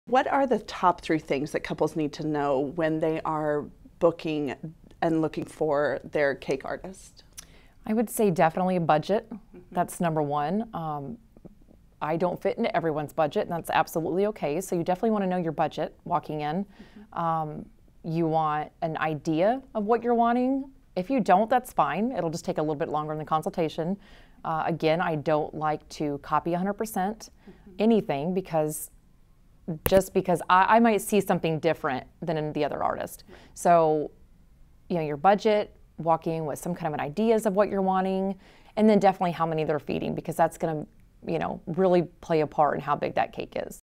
Charity Fent Cake Design Top 3
Charity Fent Cake Design
Top 3 Things To Know About Booking Your Wedding Cake Artist